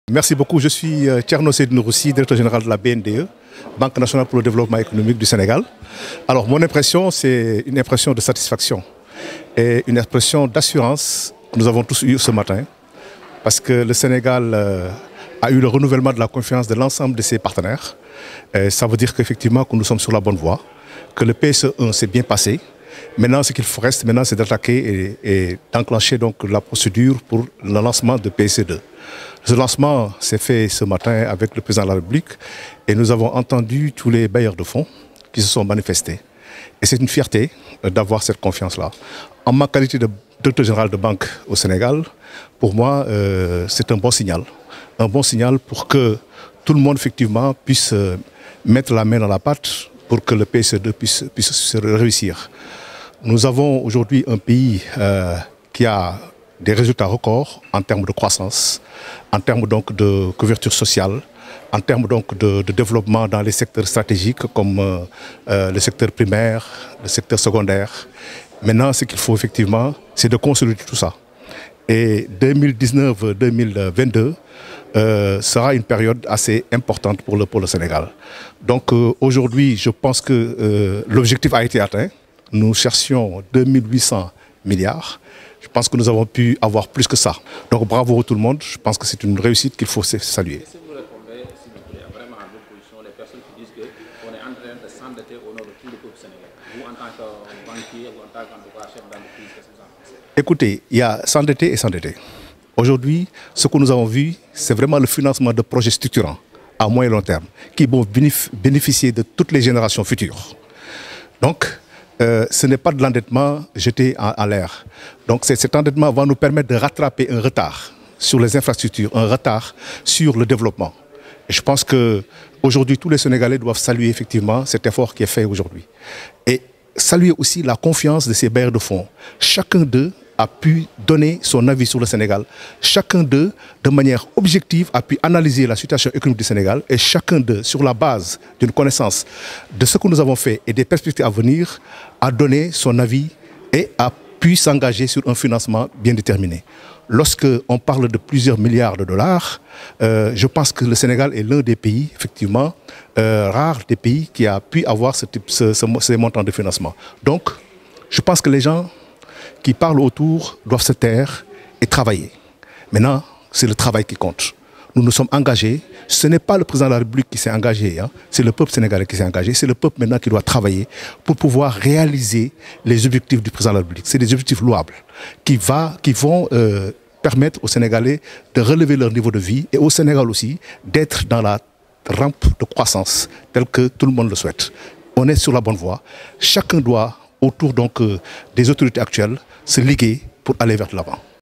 " Endettement du Sénégal", qu' en pense Thierno Nourou Sy DG BNDE
" Endettement du Sénégal": Qu' en pense Thierno Nourou Sy DG BNDS Banque Nationale pour le Développement Economique ?